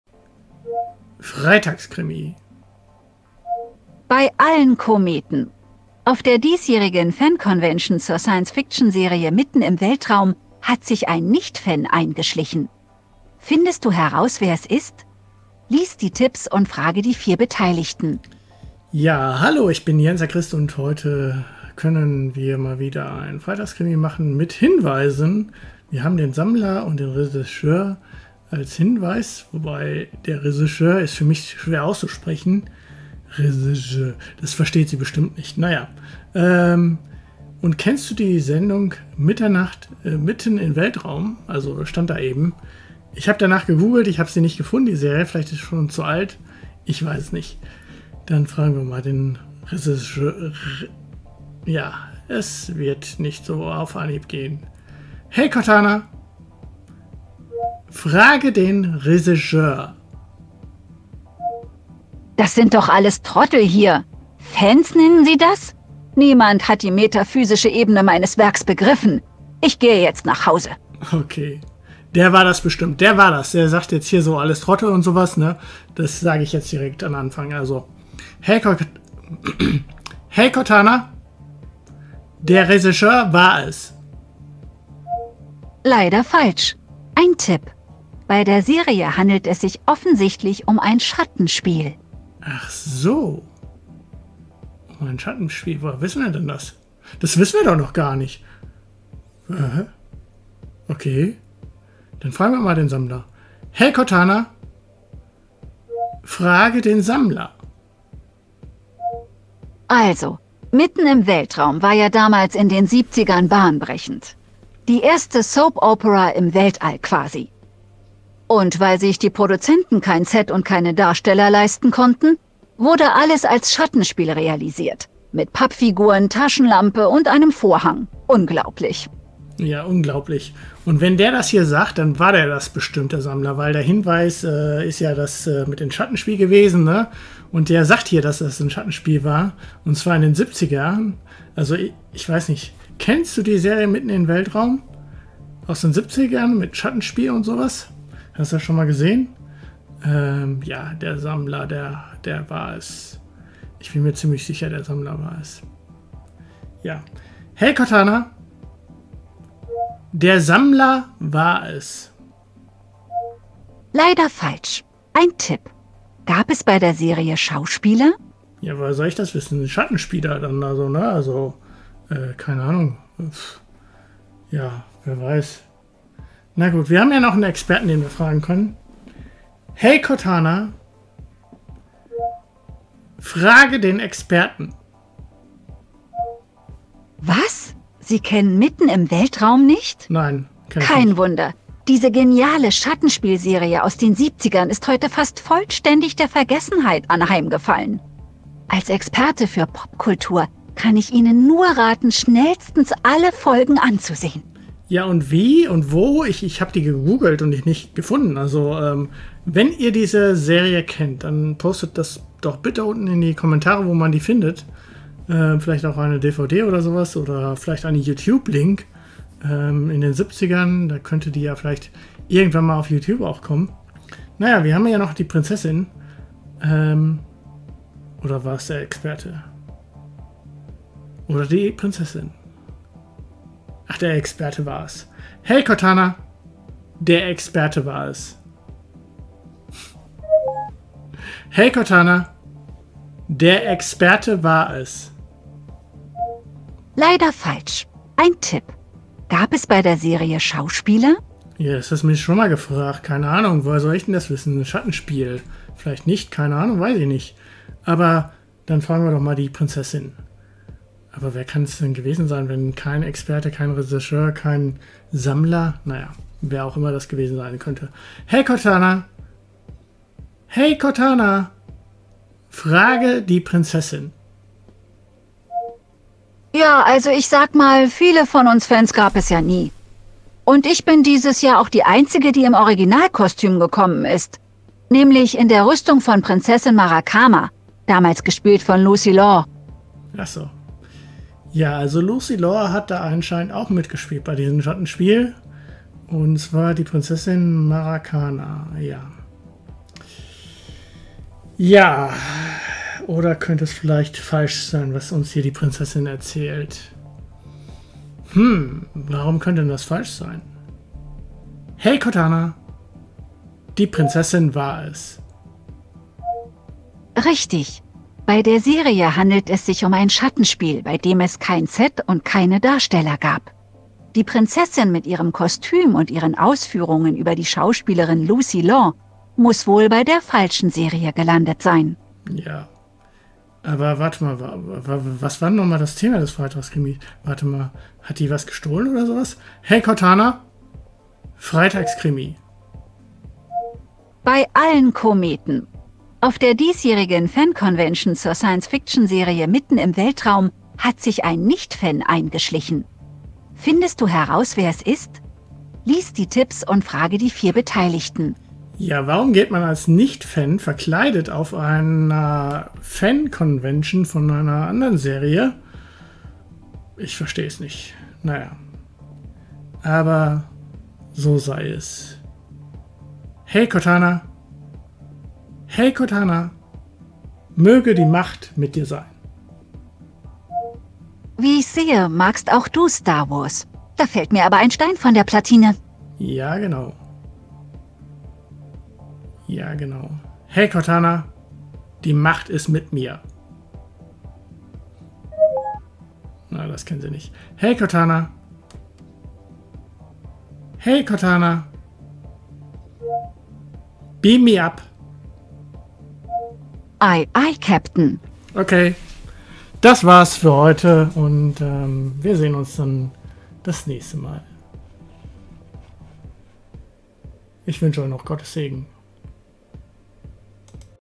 [Cortana] Freitags Krimi #028 - Science-Fiction-Serie "Mitten im Weltraum" - 24.11.2017
Bei allen Kometen! Auf der diesjährigen Fan-Convention zur Science-Fiction-Serie "Mitten im Weltraum" hat sich ein Nicht-Fan eingeschlichen. Findest du heraus, wer es ist? Lies die Tipps und frage die vier Beteiligten!

Regisseur: Das sind doch alles Trottel hier! Fans nennen Sie das? Niemand hat die metaphysische Ebene meines Werks begriffen! Ich gehe jetzt nach Hause!

Der Regisseur war es:
Leider falsch. Ein Tipp: Bei der Serie handelt es sich offensichtlich um ein Schattenspiel...

Sammler: Also, "Mitten im Weltraum" war ja damals in den 70ern bahnbrechend. Die erste Soap-Opera im Weltall, quasi. Und weil sich die Produzenten kein Set und keine Darsteller leisten konnten, wurde alles als Schattenspiel realisiert, mit Pappfiguren, Taschenlampe und einem Vorhang. Unglaublich!

Der Sammler war es:
Leider falsch. Ein Tipp: Gab es bei der Serie Schauspieler?

Experte: Was, Sie kennen "Mitten im Weltraum" nicht? Kein Wunder, diese geniale Schattenspielserie aus den 70ern ist heute fast vollständig der Vergessenheit anheimgefallen. Als Experte für Popkultur kann ich Ihnen nur raten, schnellstens alle Folgen anzusehen!

Prinzessin: Ja, also ich sag mal, viele von uns Fans gab es ja nie. Und ich bin dieses Jahr auch die einzige, die im Originalkostüm gekommen ist, nämlich in der Rüstung von Prinzessin Marakama, damals gespielt von Lucy Law.

Die Prinzessin war es:
Richtig! Bei der Serie handelt es sich um ein Schattenspiel, bei dem es kein Set und keine Darsteller gab. Die Prinzessin mit ihrem Kostüm und ihren Ausführungen über die Schauspielerin "Lucy Law" muss wohl bei der falschen Serie gelandet sein!